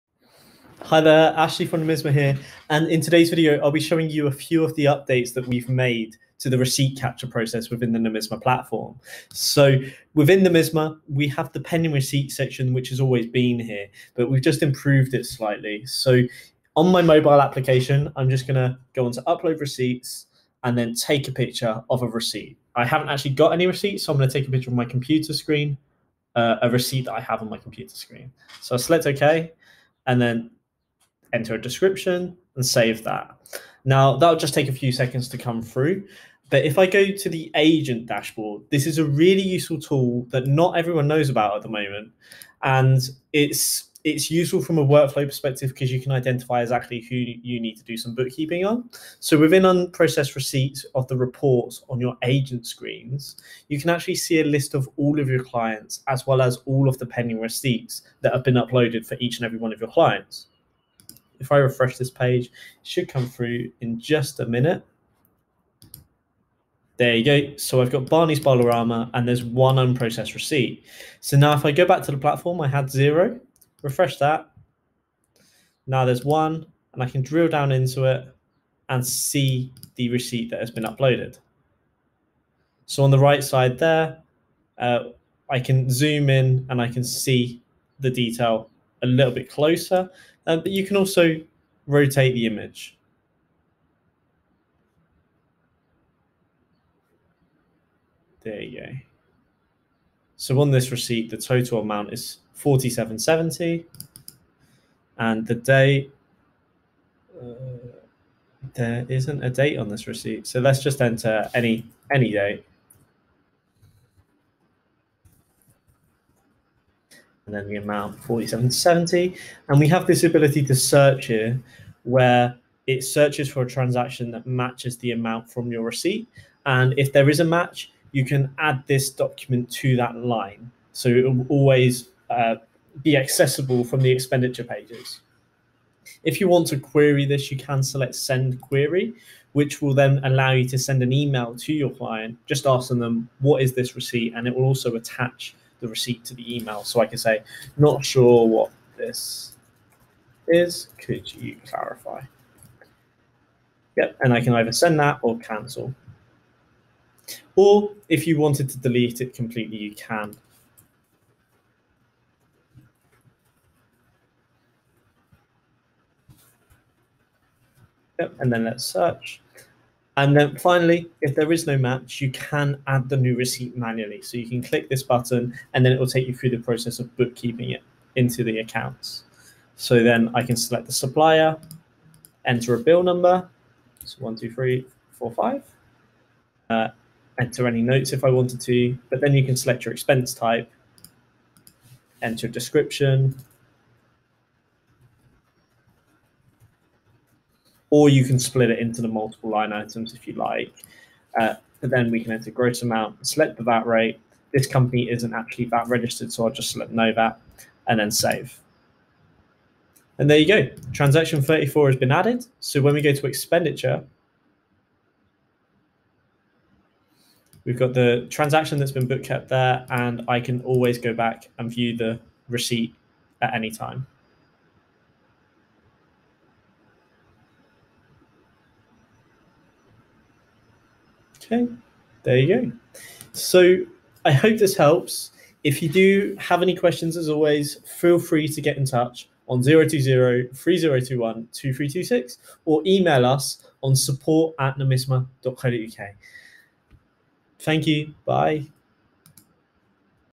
How to upload and bookkeep images of receipts on Nomisma
In this video we cover how to manage receipt workflow, upload receipts and bookkeep them using the Nomisma platform.

The mobile receipt capture app is included within all bookkeeping packages. If you'd like to find out more, do get in touch: